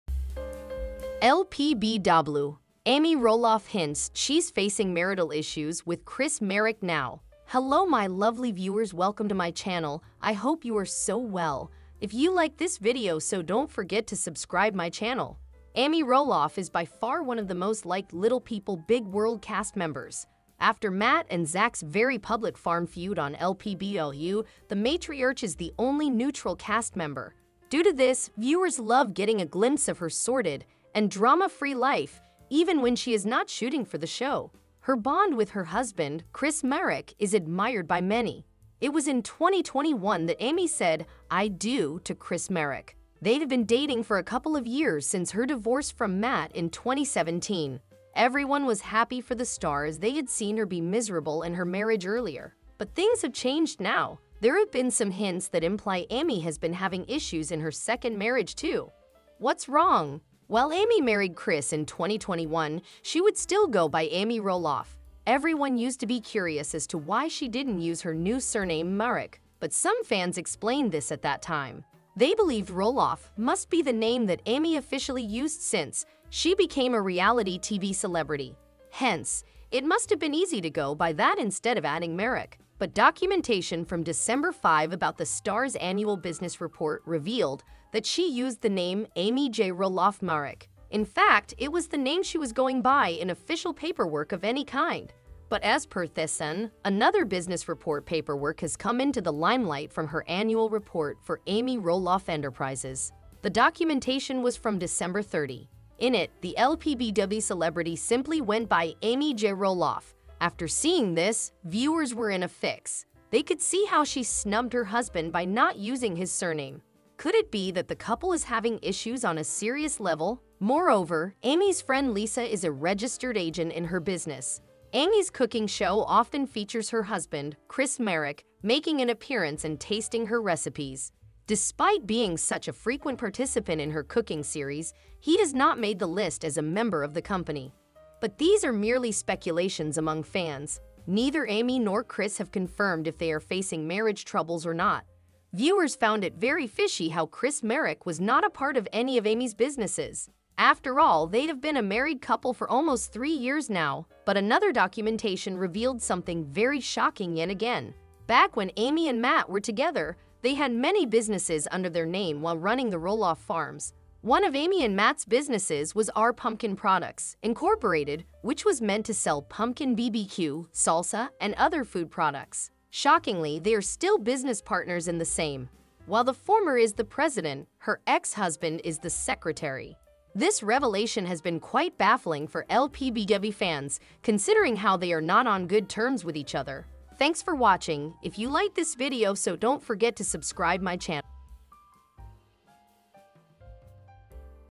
Marital Issues! Amy Roloff And Chris Marek Facing Of Divorce | It Will Shock You
LPBW Amy Roloff Hints She’s Facing Marital Issues With Chris Marek Now

Affable America"  is a channel where you will find all about trending celebrity gossip. We try our best to give our audience the most truthful Gossip.